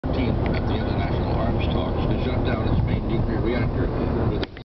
Crazy mouse house on Elvis Presley Blvd.
From the WTF files, we passed this crazy place heading south on Elvis Presley Blvd. on the way to Graceland.